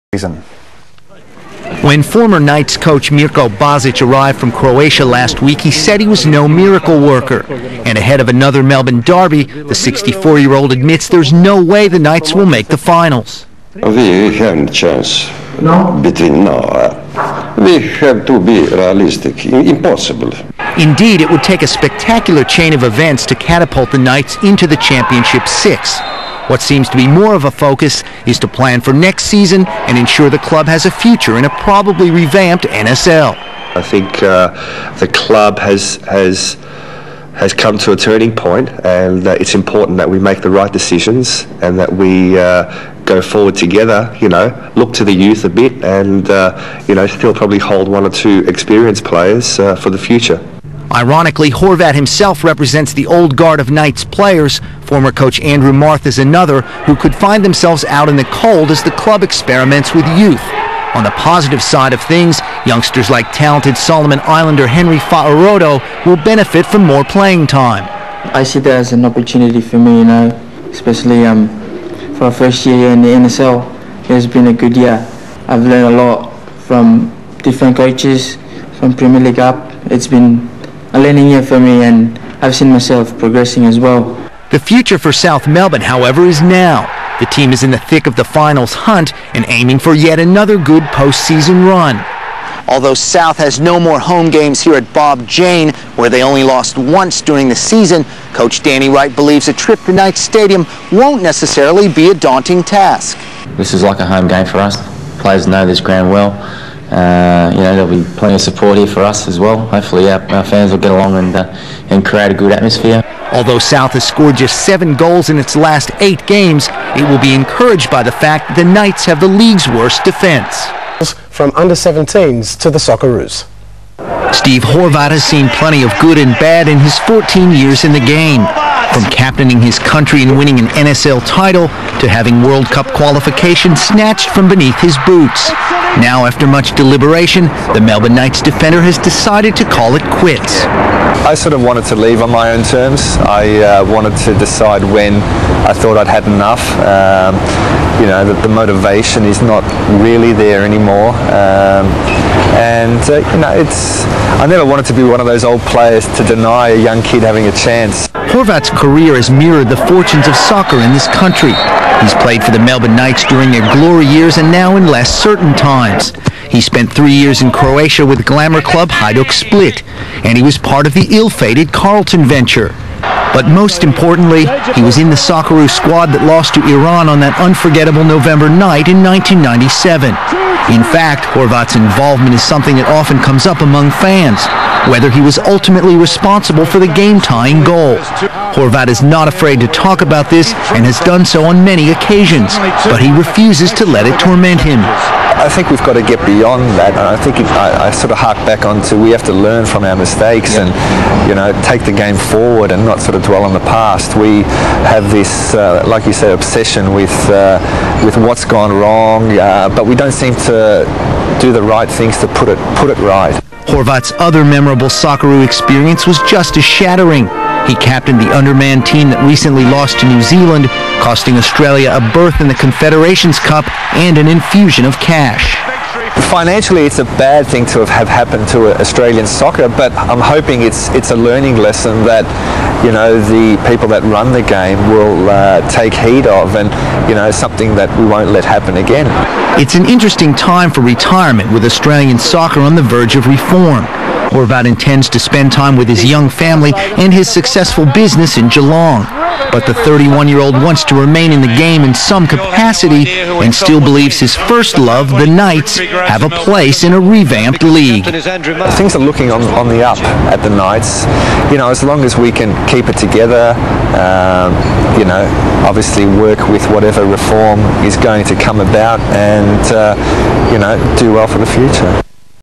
Melbourne Knights-Mirko Bazic 3
Bazic leads the knights charge against south